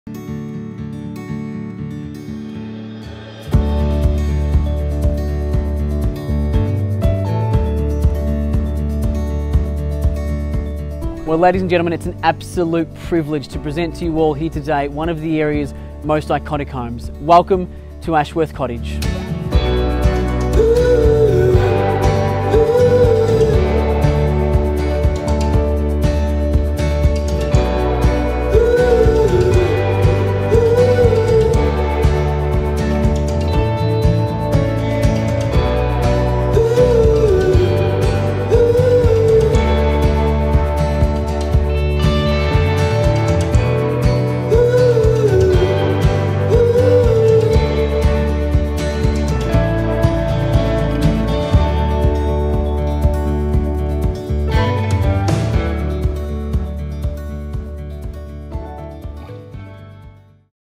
20 Lower Road, Wattle Glen
Cottage style living surrounded by Wattle Glen's renowned greenery.